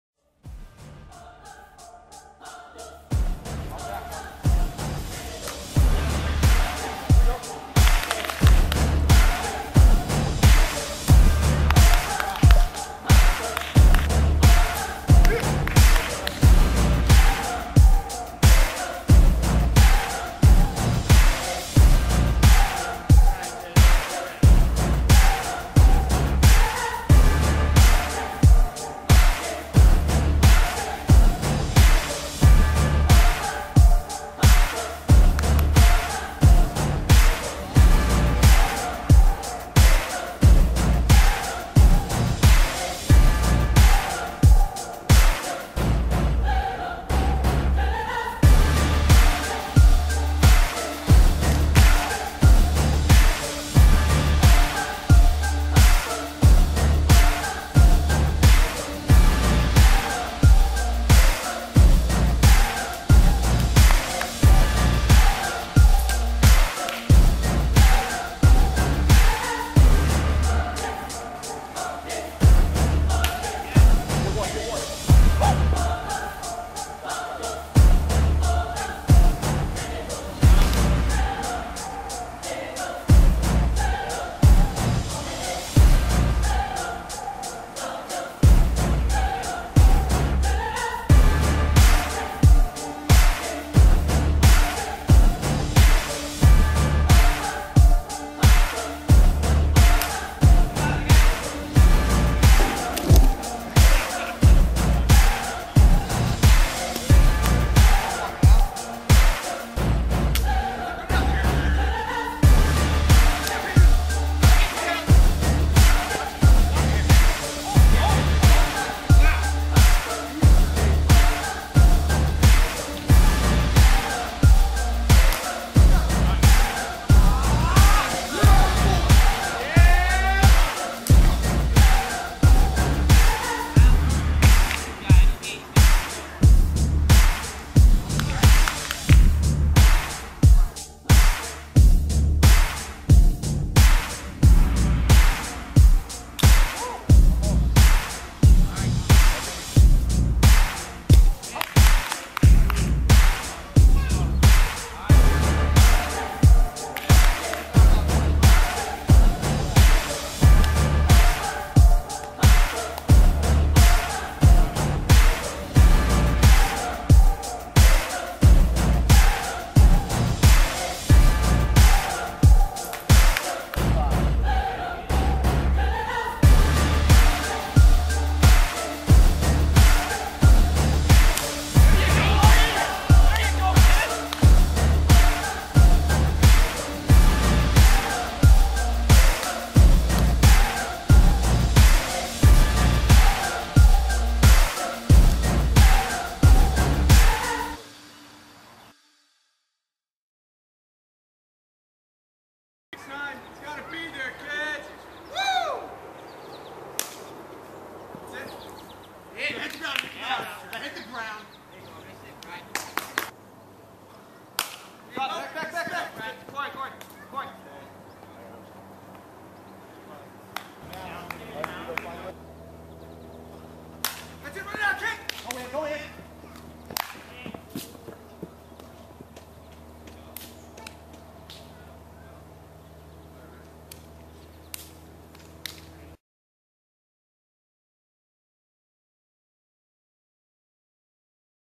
2011 Playoffs: Tigers vs Indians
The Indians play host to the Tigers. Game one of the 2011 HLDS.